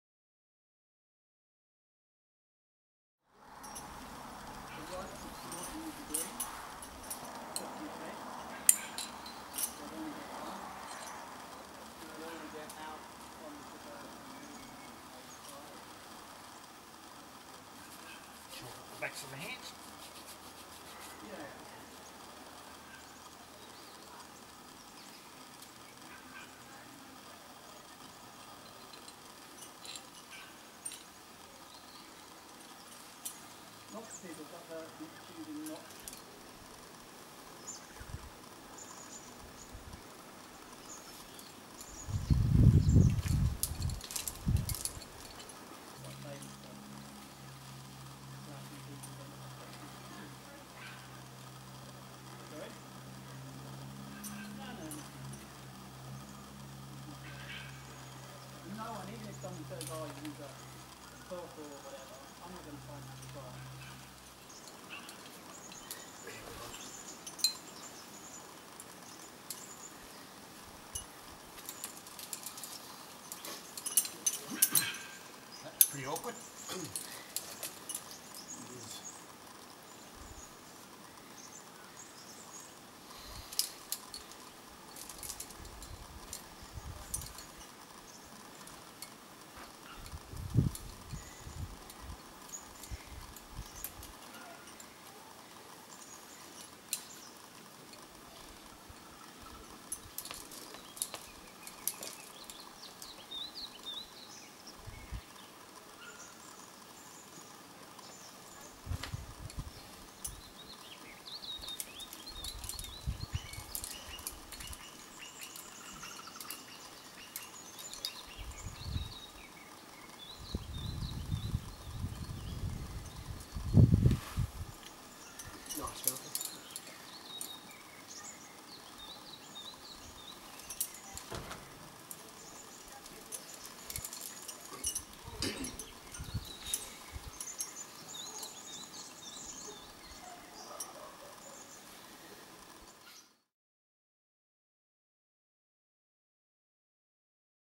Malcolm Matheson on Kryptonite Crack, 23 at Mt Arapiles
So I guess you face climbers fresh off Lord of the Rings are watching this video and thinking, "golly, that crack climbing don't look all that jolly difficult - even that old man is doing it with apparent ease". Well firstly, who talks like that? Stop it! And secondly, go jump on Kryptonite Crack and let's see if you don't find yourself exclaiming "blimey this jamming nonsense is quite difficult, and it hurts my hands, lets pop down to Nati Cafe for a fresh soy latte instead!". Wow, you face climbers talk funny.